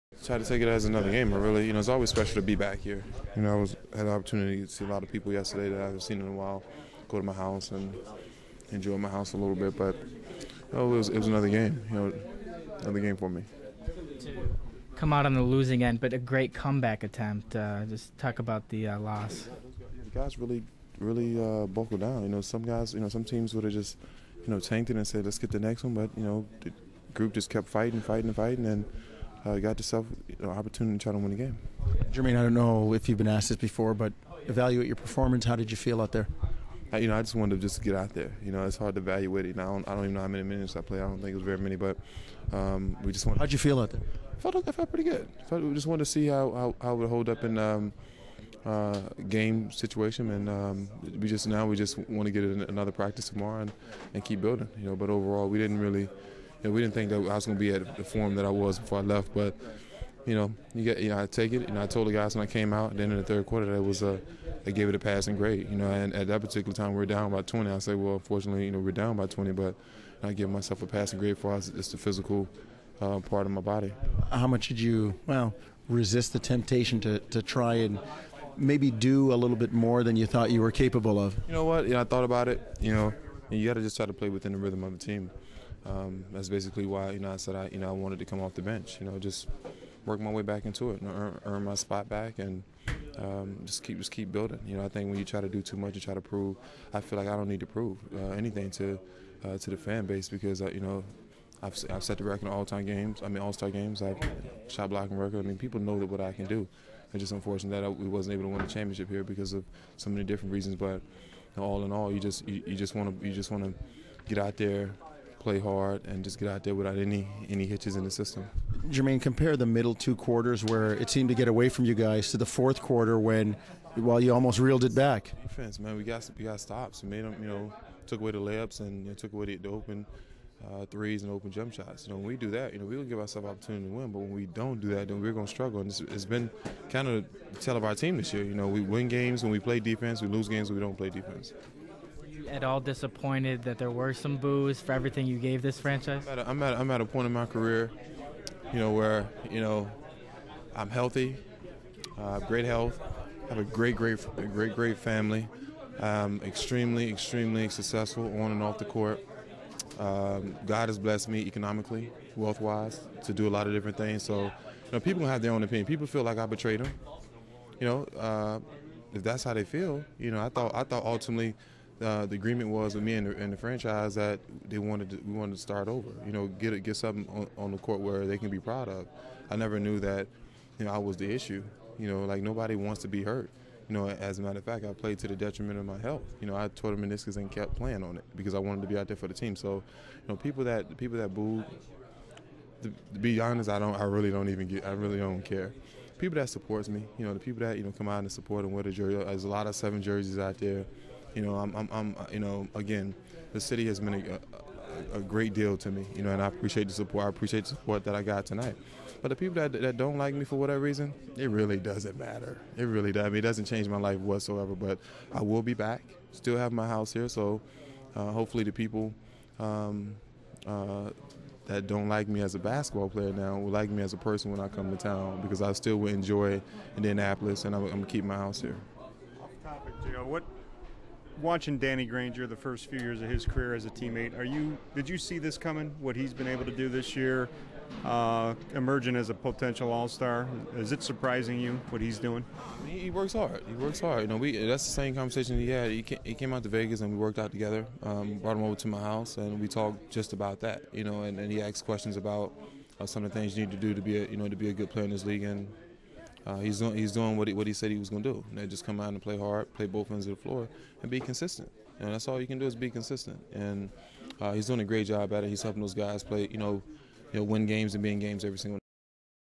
Jan. 16/09 - Raptors vs Pacers - Jermaine O'Neal
Post-game interview with Jermaine O'Neal.